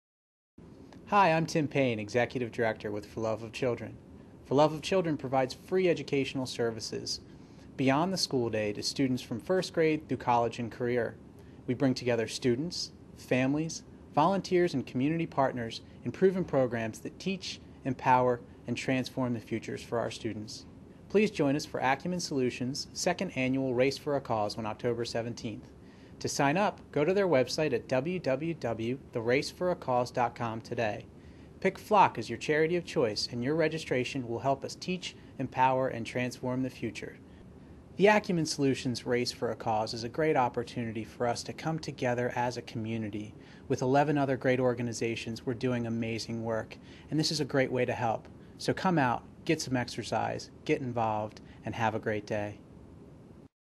Join FLOC in the Acumen Solutions Race for a Cause 8k - Oct 17th
So, How can YOU help?
1)     Sign yourself up for the race

2)     Get friends, colleagues and family to sign up

3)     Help us spread the word to the world